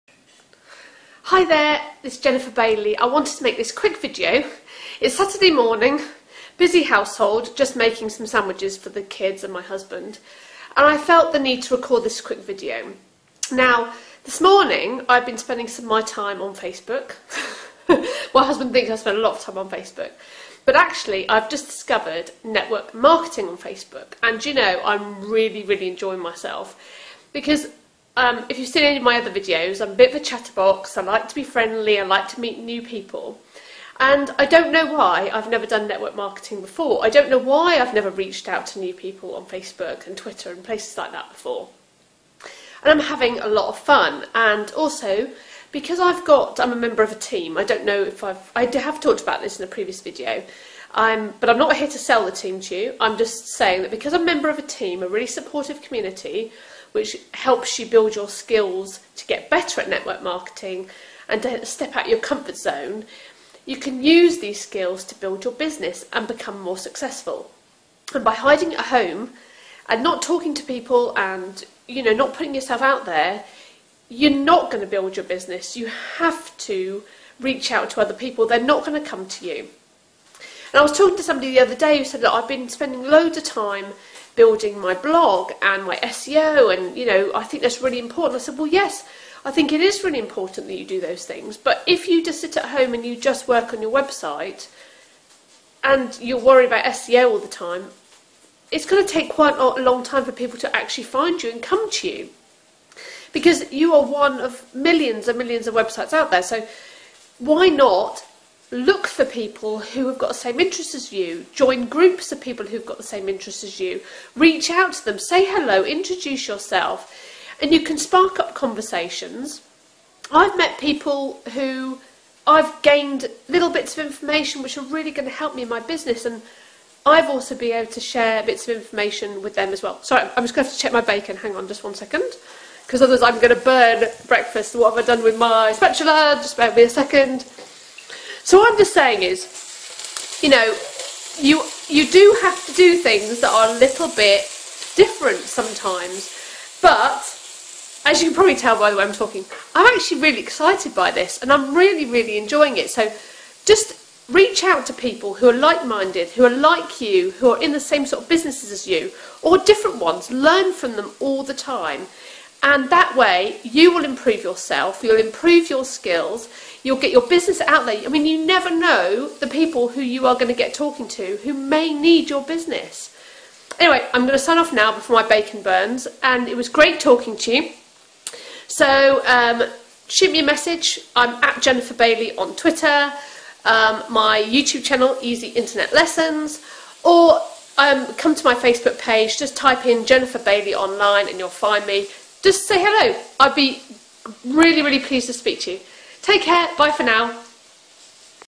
Networking - Why you should do it!
This is a video where I talk about discovering networking and why you should do it. It is important to reach out and connect with people.

By the way I didn't burn the bacon!